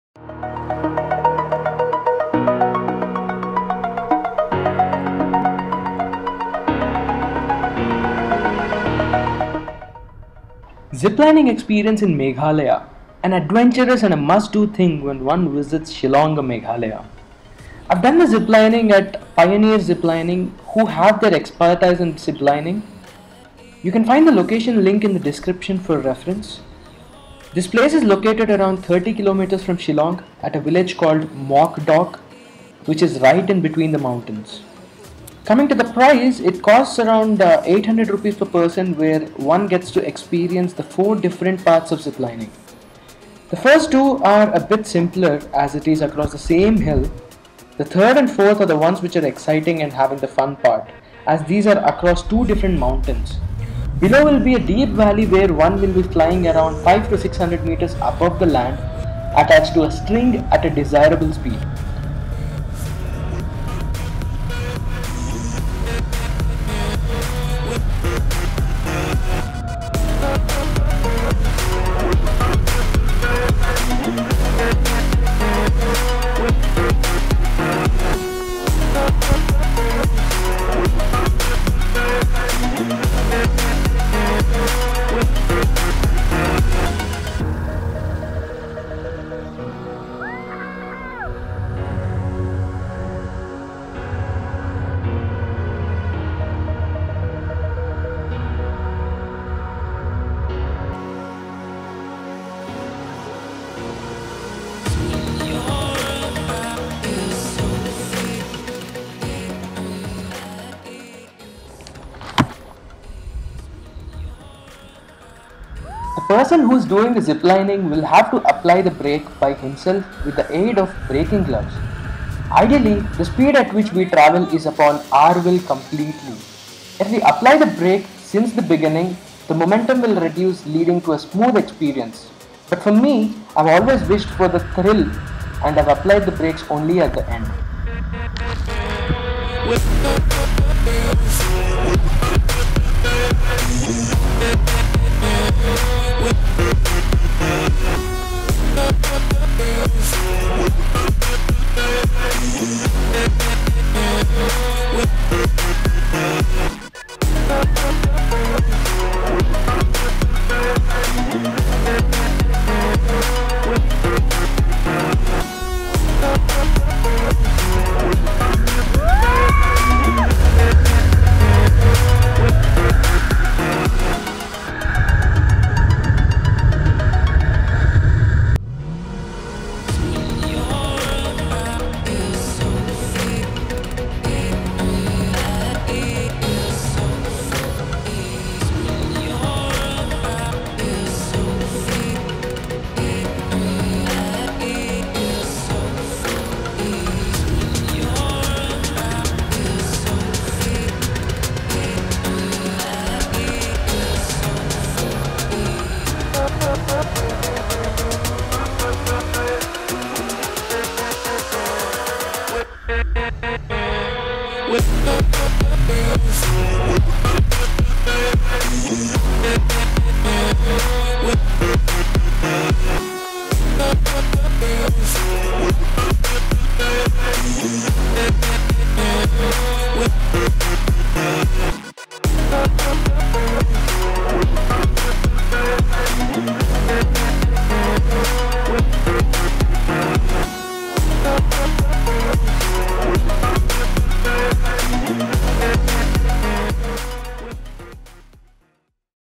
Zip Lining Experience in Meghalaya - One of the best
I have been to Meghalaya for a 5 day trip with Goaventureofficial and here is a glimpse of my Meghalaya adventure where i got to experience the Zip lining which was one of its best.

Price - Rs.800 per pax (group discounts were also available)